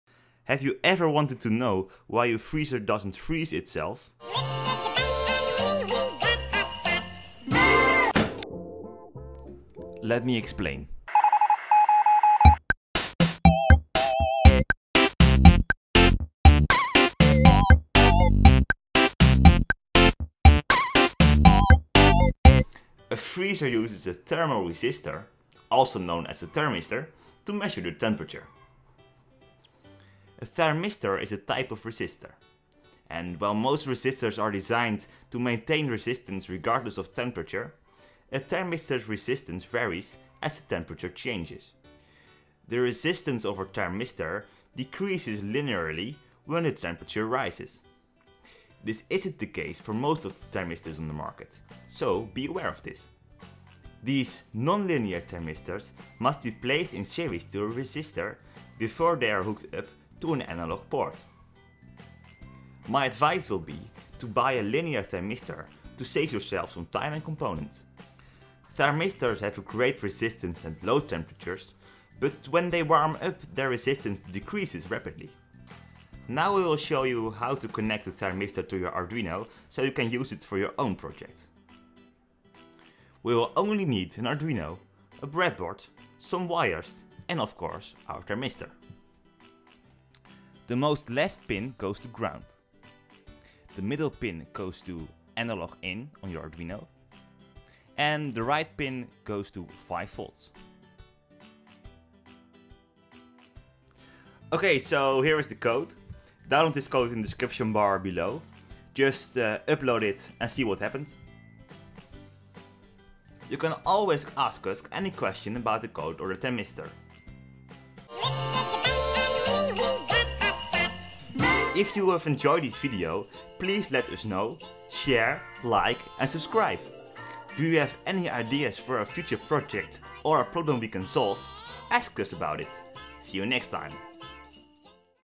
Thermistors | How HOT is your Robot? - Mike's Lab
In this episode we're exploring the Thermistor
URL to download the code:

Thanks Audiomicro for the music!